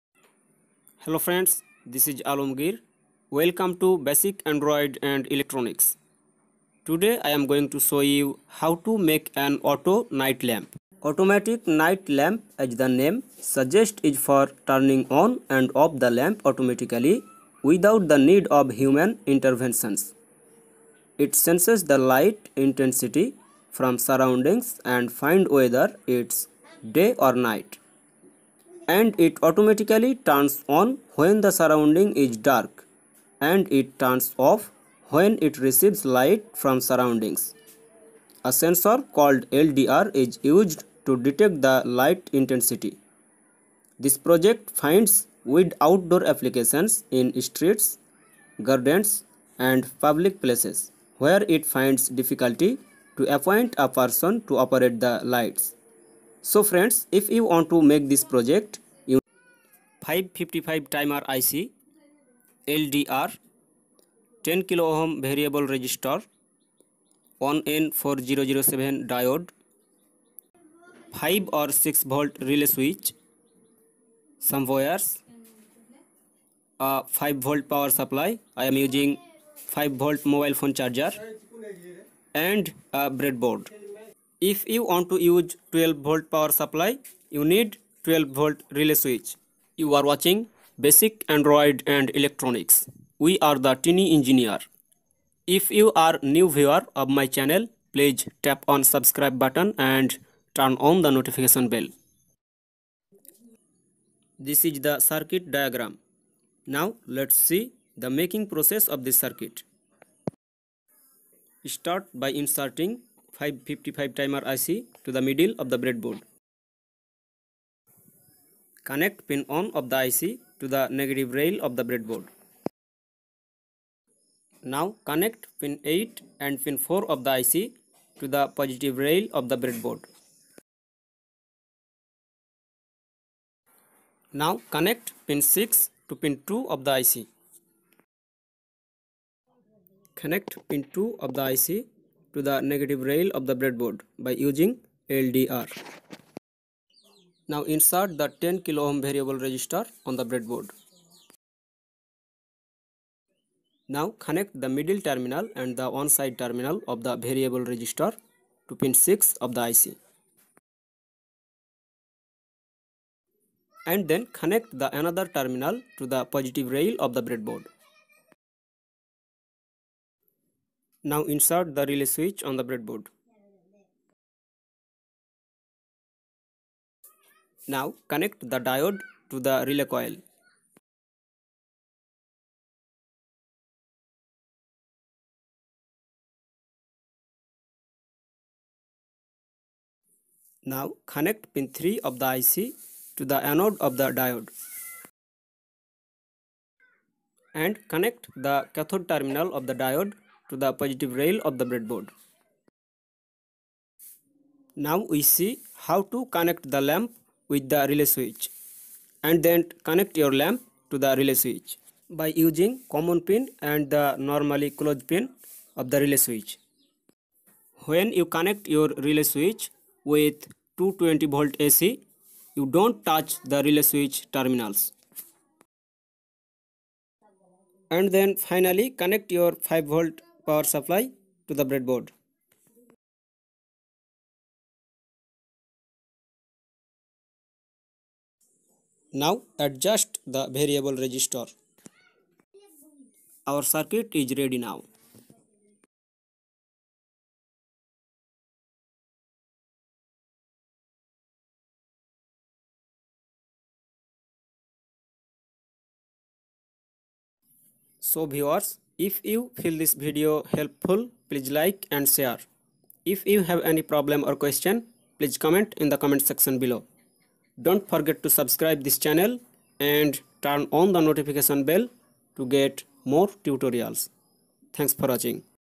How To Make An Auto(on/off) Night Light Circuit On Breadboard
In this video I shared with you How to Make An Auto Night Light Circuit on Breadboard at Home. To make an auto night light circuit on breadboard you will need➡️

1. NE555 Timer IC,
2. LDR,
3. 10 KΩ Variable Resistor,
4. 1N4007 Diode,
5. 5 or 6 Volt Relay Switch,
6. Some Wires,
7. A 5 Volt Power Supply and
8. a Breadboard.

The process of making the auto night light circuit and the circuit diagram of this project showed in this video tutorial. So if you want to make the auto night light circuit on breadboard at home just collect the components and watch the video tutorial carefully. After watching the video try to make the auto night light circuit on breadboard.